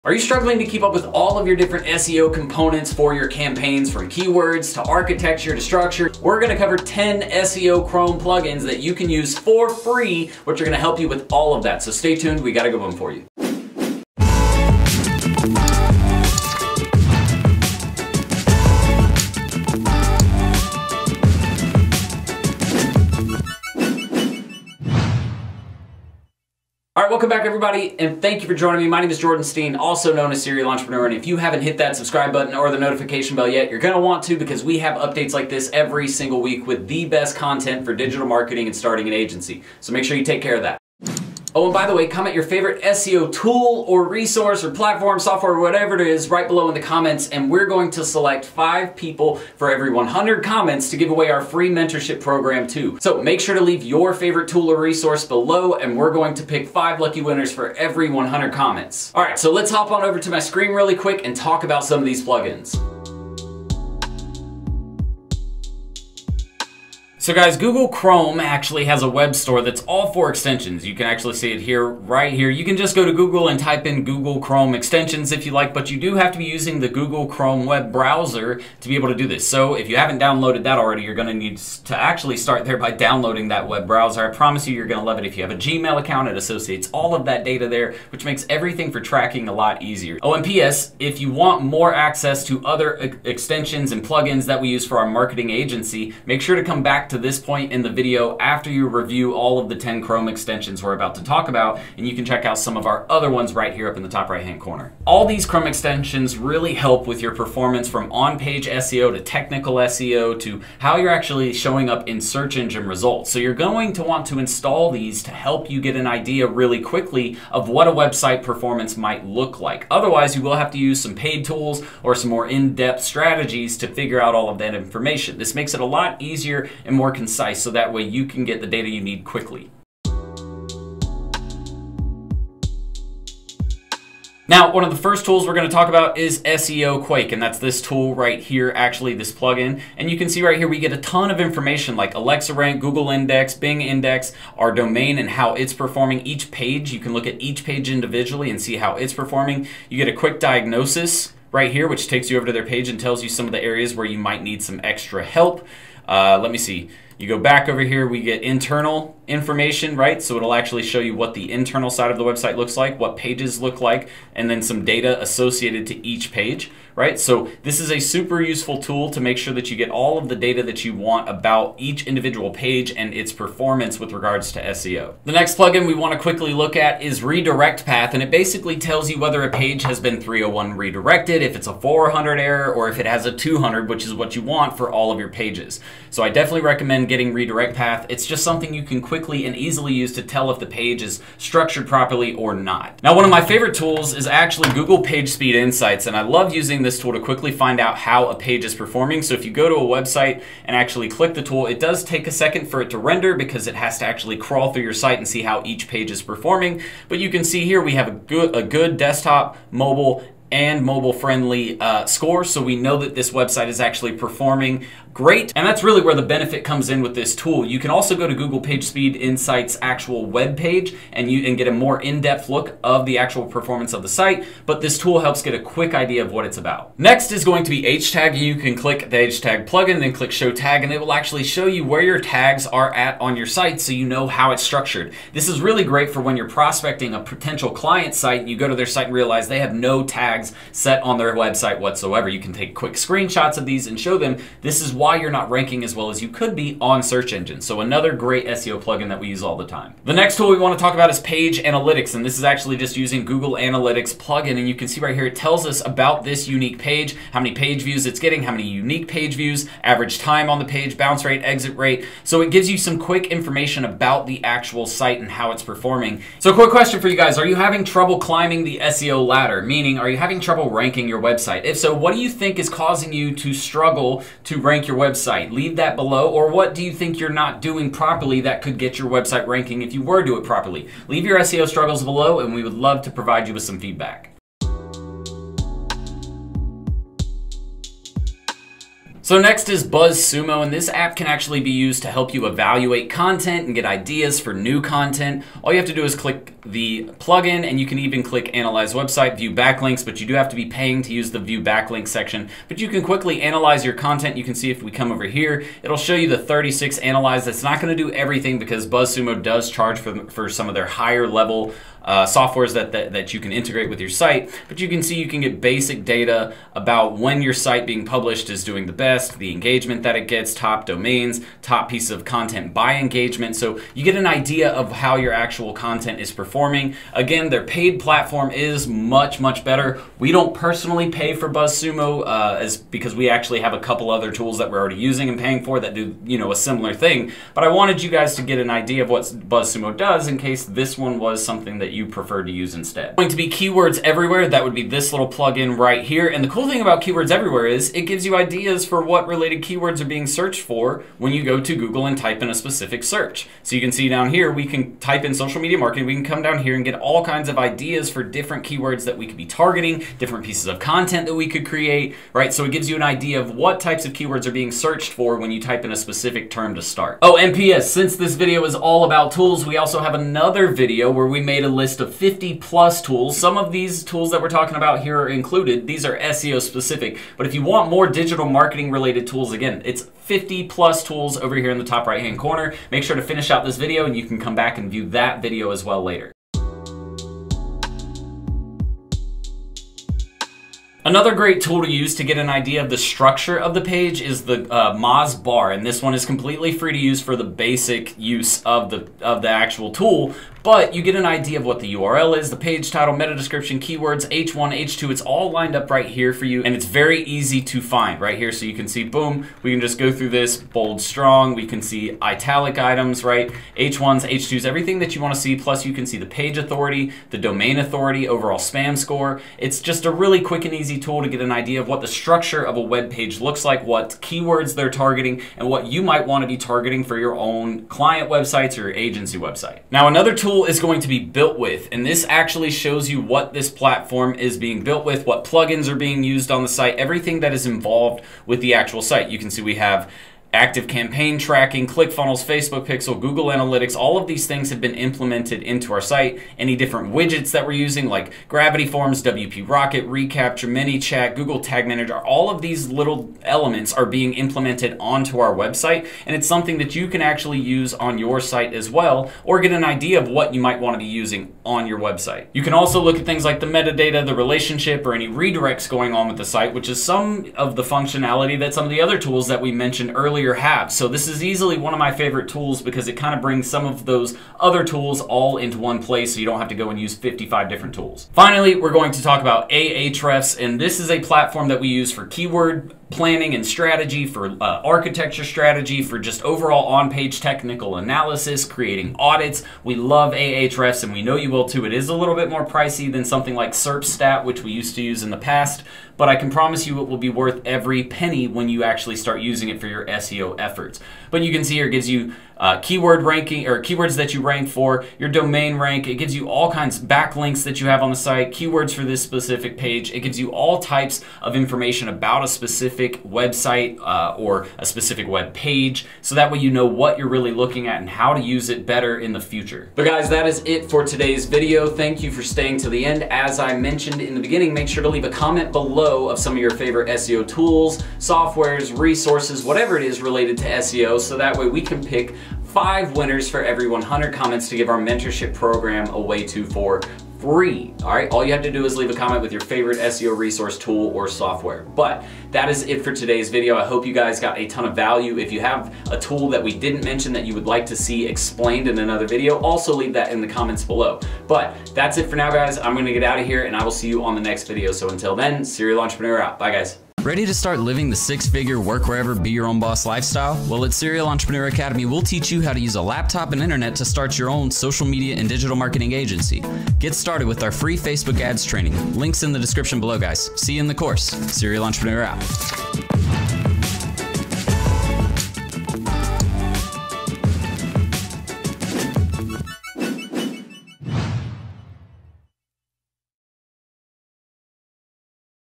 Top 10 SEO Chrome Extensions for Marketing Agencies in 2020
Want to unlock the SEO power of your Google Chrome browser, with FREE Chrome extensions? Well, today we’re going to run down the top SEO Chrome extensions for marketing agencies in 2020, and how to get the must-have Chrome extensions your SMMA needs!

“Extensions” are add-ons to the Google Chrome interface that can maximize the functionality of your browser. SEOChromeExtensions immediately gather and present SEO data to help you fix simple to complex problems. Properly used, installing these cool Chrome extensions can improve your productivity, and help you organize your SEO process seamlessly. To help assess which SEO Chrome extension(s) can help your DigitalMarketingAgency the MOST, we have to break our Top 10 list into different categories, based on their purpose: On-Page SEO, SEO Strategy, and Competitor Research.

THE BEST ONPAGE SEO CHROME EXTENSIONS
- SEOQuake

CHAPTERS
PART 1 - The Best On-Page SEO Chrome Extensions - 00:02:04
1) SEOQuake - 00:02:12
2) Redirect Path - 00:02:52
3) Google PageSpeed Insights - 00:03:10
4) H-Tag - 00:03:46
5) Page Analytics - 00:04:06

PART 2 - The Best SEO Strategy Chrome Extensions - 00:04:39
6) Buzzsumo- 00:04:48
7) Keywords Everywhere - 00:05:40

PART 3 - The Best Competitor Research Chrome Extensions - 00:06:21
8) MozBar - 00:06:31
9) Builtwith - 00:07:05
 10) Ahrefs - 00:07:55